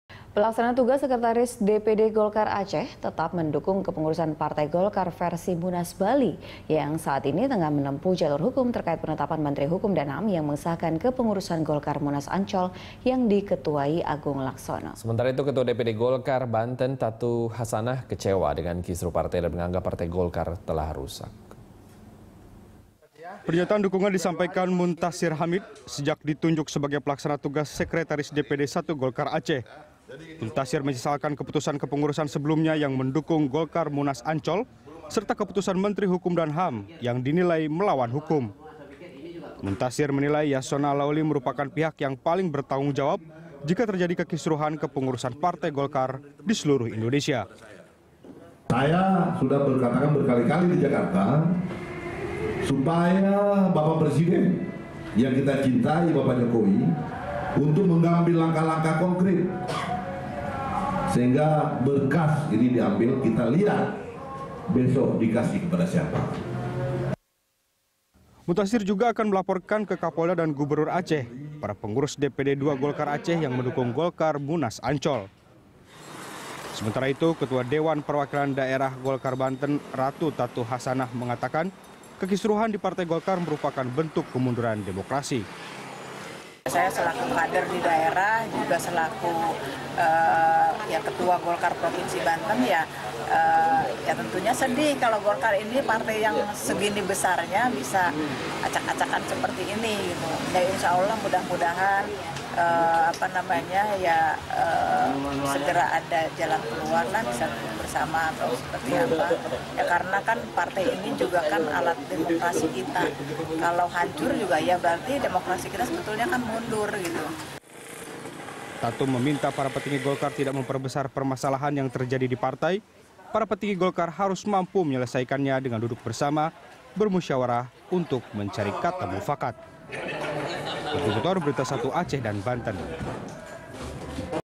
Ratu Tatu Chasanah: Saya Sedih Melihat Golkar Acak-Acakan
Pelaksana Tugas Sekretaris DPD Golkar Aceh tetap mendukung kepengurusan Partai Golkar versi munas Bali yang diketuai Aburizal Bakrie. Sementara itu Ketua DPD Golkar Banten Ratu Tatu Chasanah kecewa dengan kisruh partainya dan menganggap partai Golkar telah rusak.

Facebook.com/BeritaSatuTV
Youtube.com/BeritaSatu